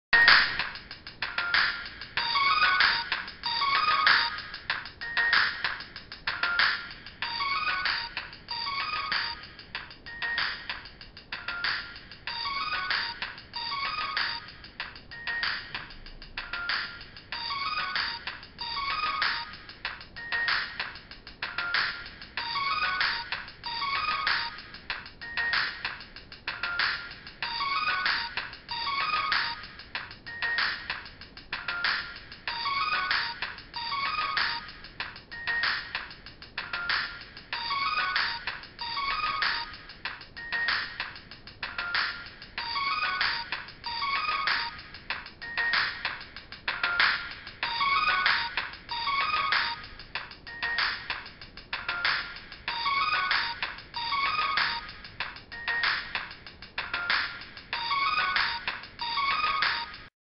Freestyle beat on my ipod touch
A beat made on my ipod touch using beat maker app